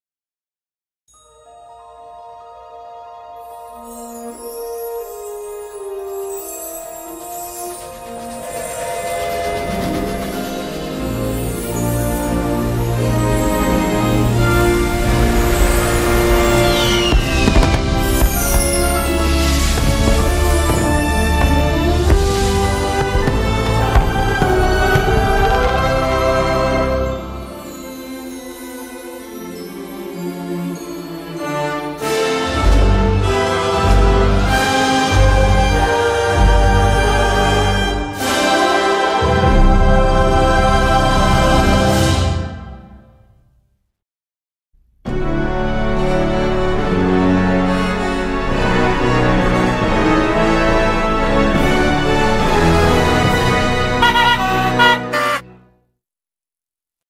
Disney/Lord Miller Productions (TBA)
For @alexthetetrisfan1064 and @SpongeBobFanatic2K6

What if Lord Miller Productions made a partnership with The Walt Disney Company?